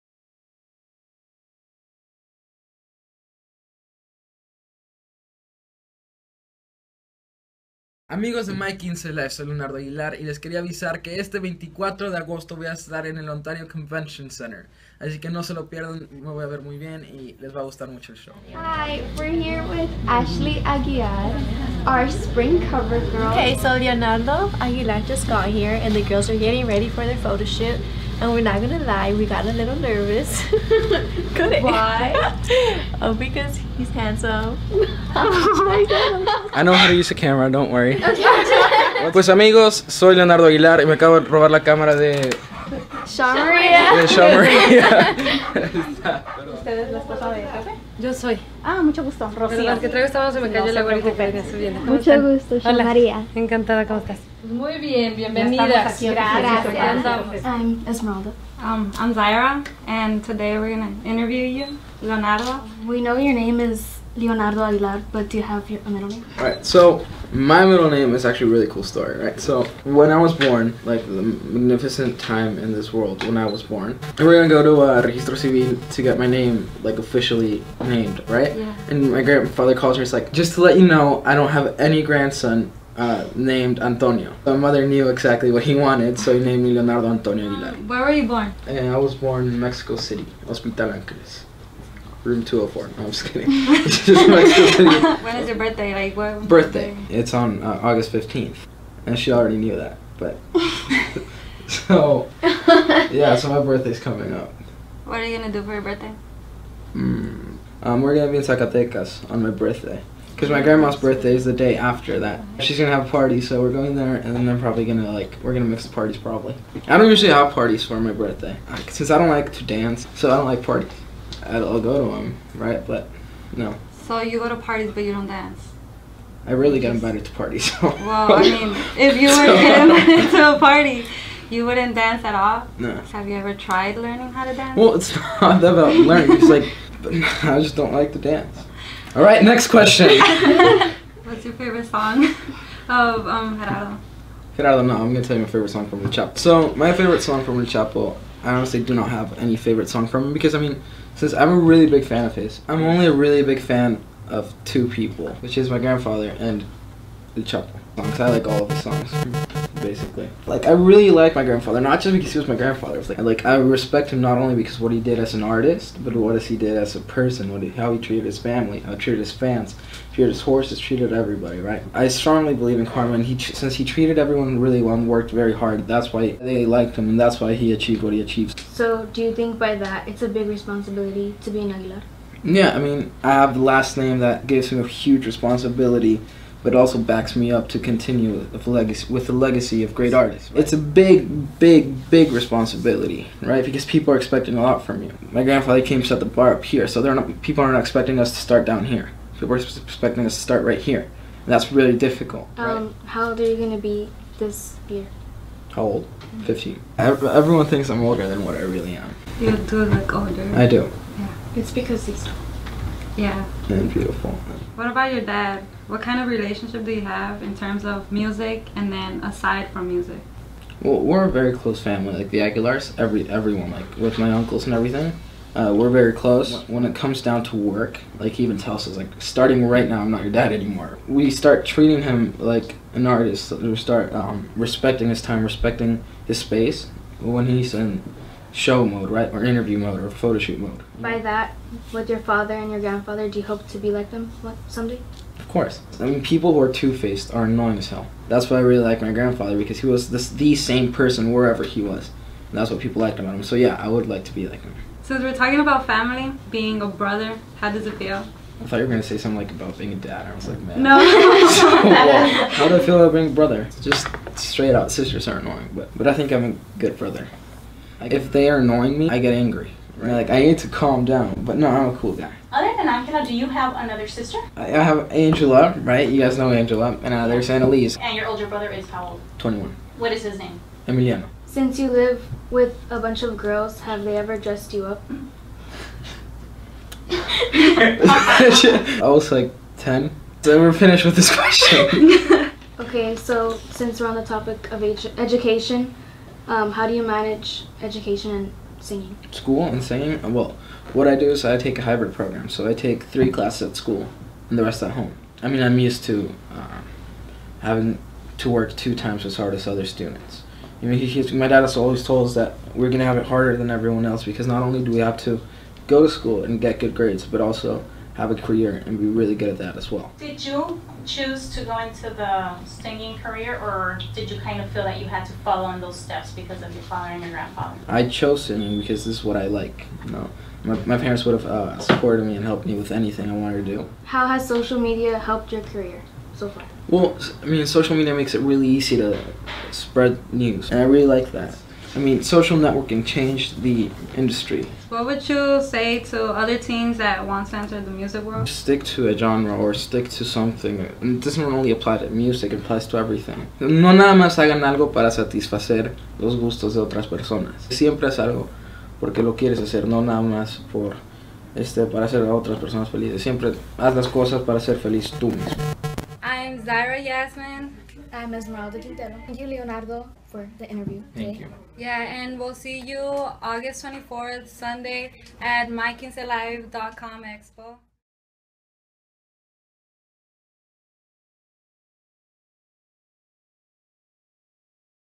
MyQuinceLife Interview with Leonardo Aguilar
See you at our Quinceanera EXPO.
A portion of the net proceeds will be donated to Children's Hospital Los Angeles

Video edited by MyQuinceLife.com Teen Writer: Sham!

About the video: MyQuinceLife.com Teen Writers, Cover Girl Finalists and Friends interview emerging teen artist Leonardo Aguilar (La Nueva Generacion de la Familia Aguilar).